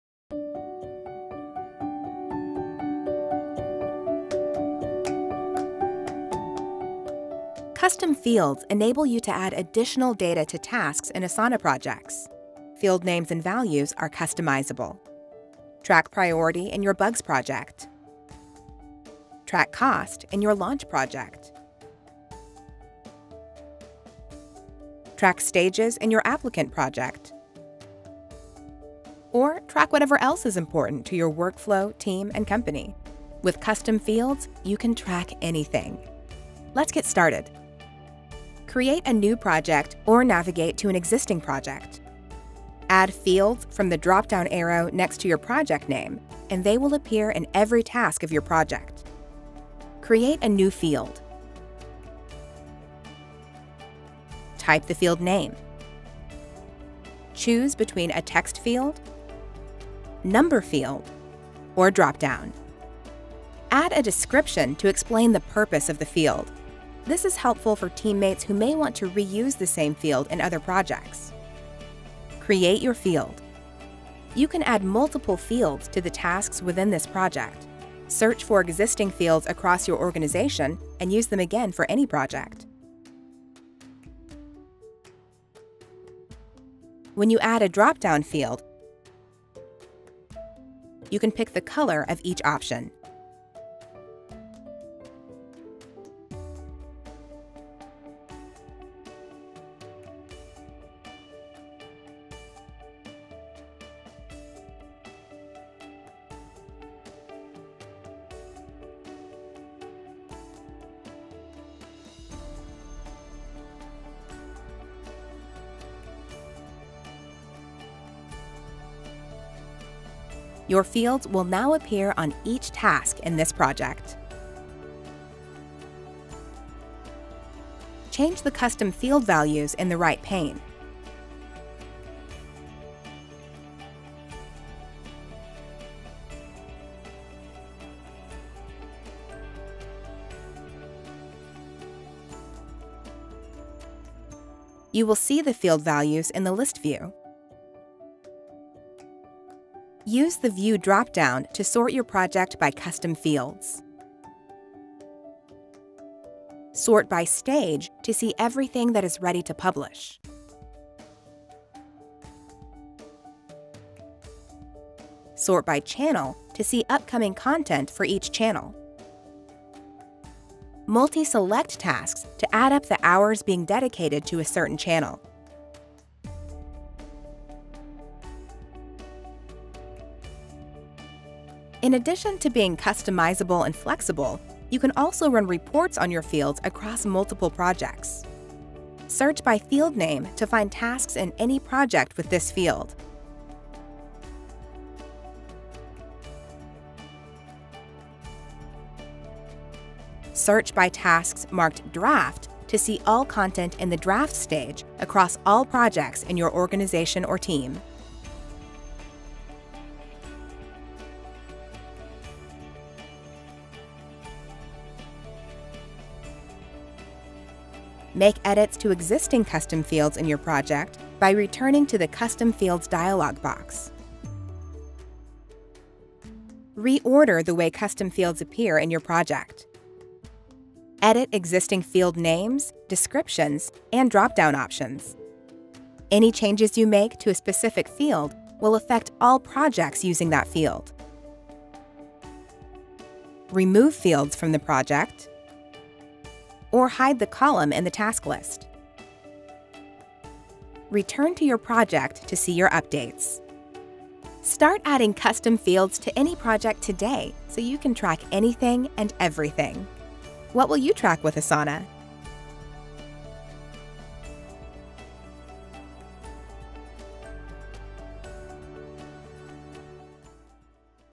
Using Custom Fields to track anything | Asana tutorial
Track anything with Asana's custom fields feature. This allows you to search by field name and run custom reports according to custom tags. WIth customizable fields, you can organize projects and tasks in any way you need to keep your team organized.

Every organization is unique and has it’s own type of work it needs to track. With custom fields, each organization can decide what matters to them, and track that directly in Asana.

Use fields to plan, prioritize, manage and track all of your work, from start to finish. Track the parts of a project that are most important to your team/organization. Easily add, edit, and update fields, so everyone knows where things stand at any point in time.

Custom fields expand Asana from simple task management, to a tool that can capture all of your team’s workflows and be a source of truth for your organization. There is no limit to the fields you can create, making it simple to track and manage anything and everything that’s important to your organization